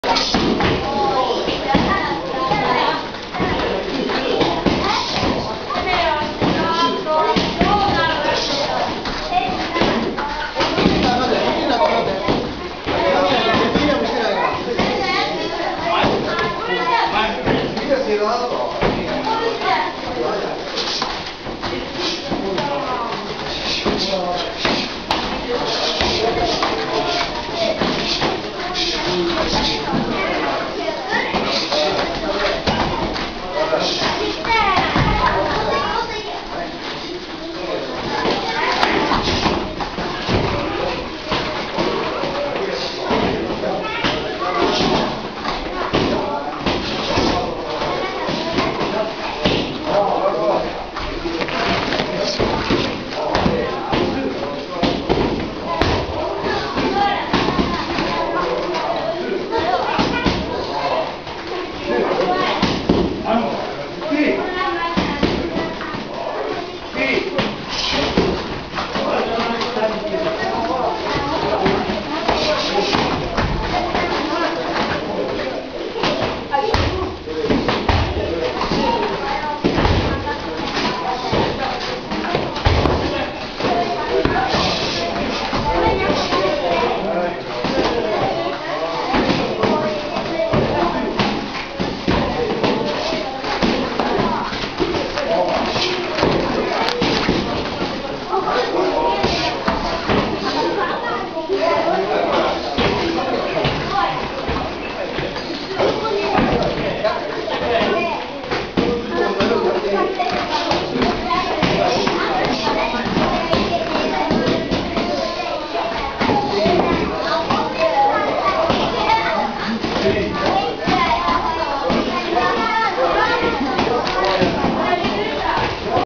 マススパ１.AVI
M-BLOWに出稽古して来た時のマススパの動画です。